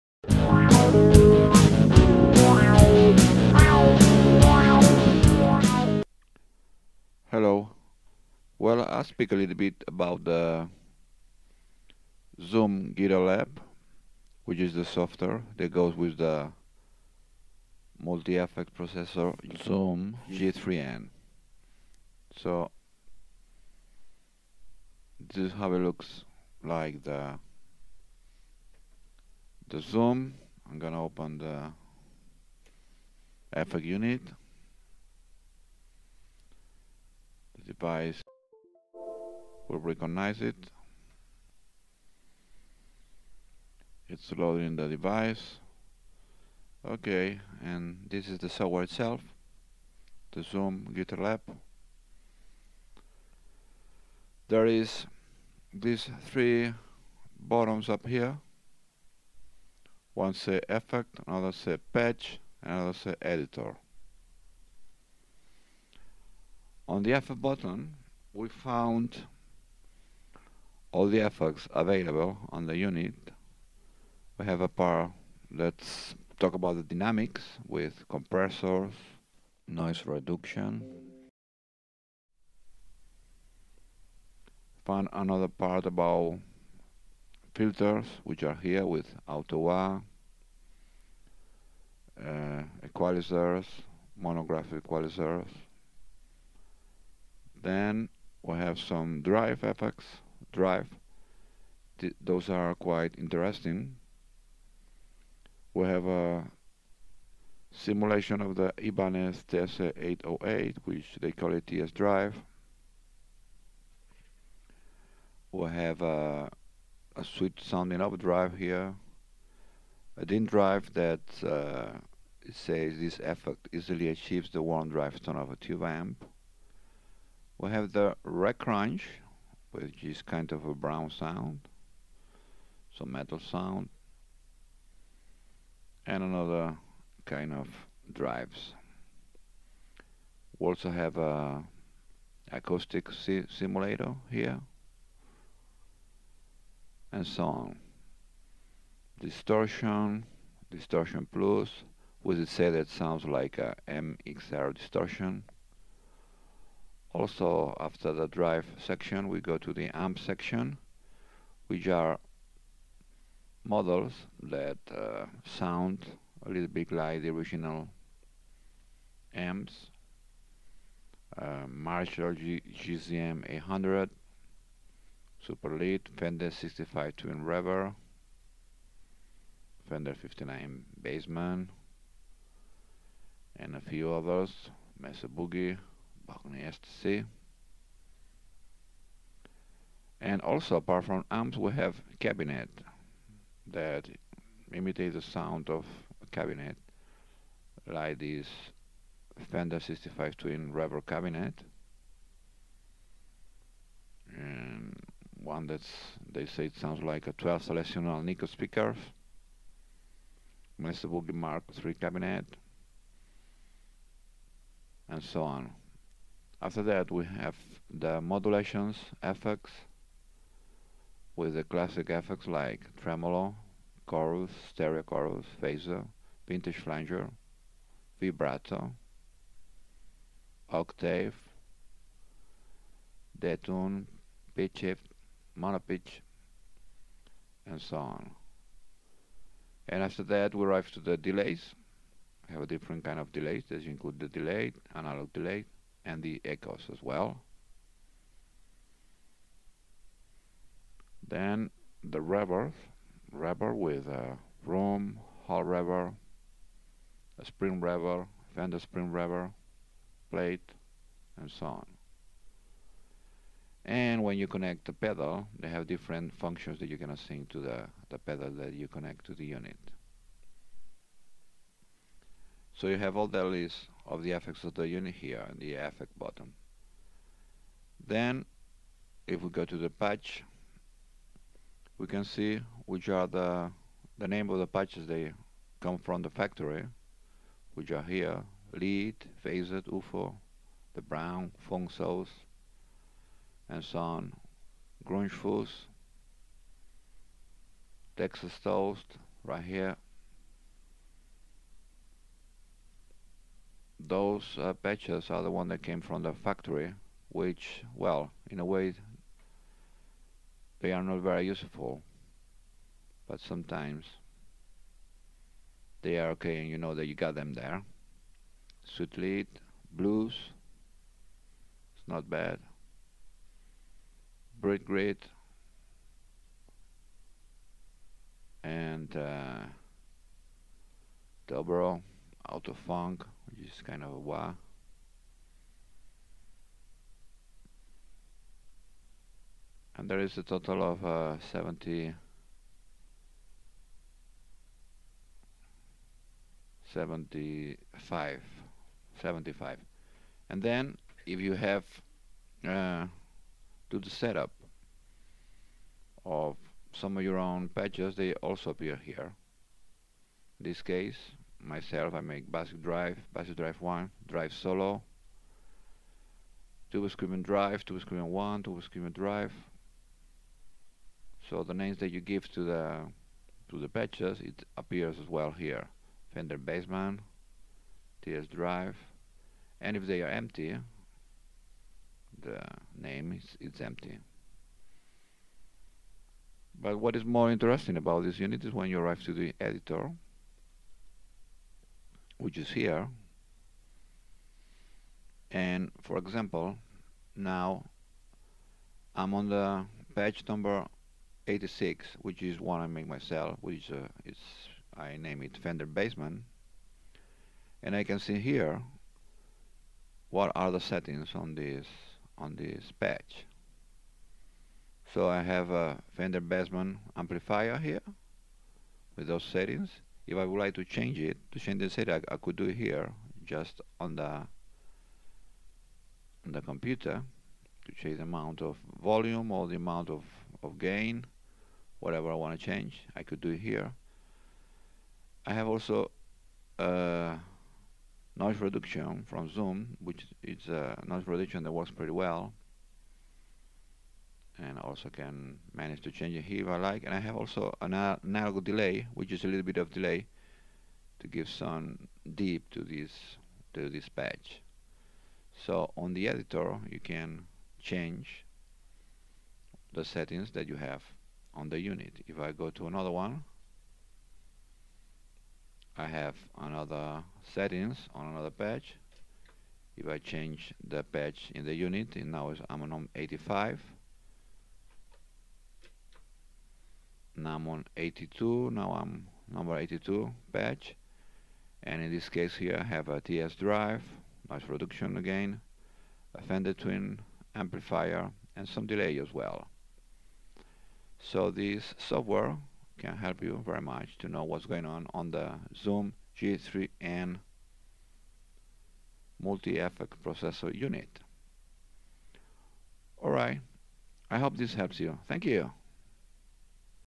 Zoom Guitar Lab software & Zoom G3n guitar effects processor.
Quick view of the software Zoom Guitar Lab with wich to edit throught a computer the guitar effects Zoom G3n and other Zoom guitar effects.